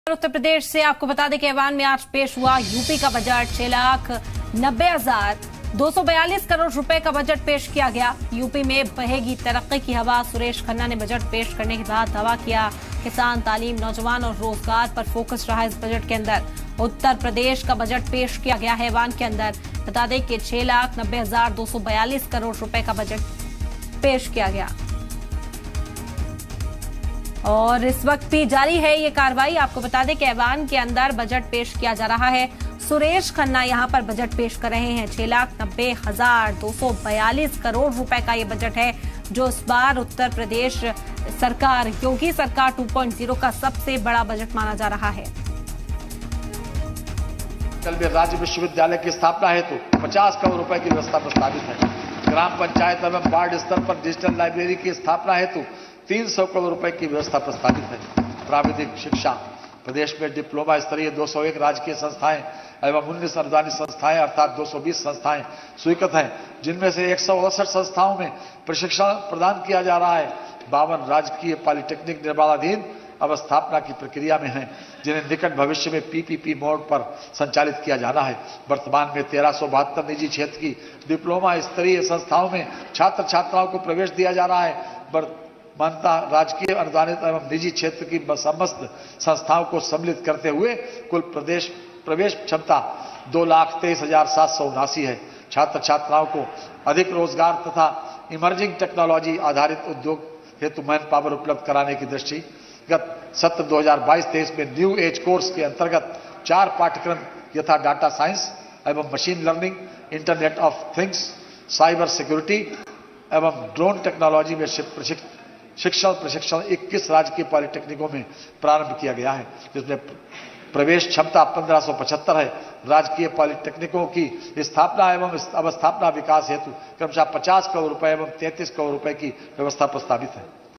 Uttar Pradesh Budget 2023 | असेंबली में पेश हुआ यूपी का बजट | Lucknow | CM Yogi | BJP | Urdu News
असेंबली में पेश हुआ यूपी का बजट, 6 लाख 90 हजार 242 करोड़ रुपये का पेश किया गया बजट

UP's budget presented in the assembly, a budget of Rs 6 lakh 90 thousand 242 crore was presented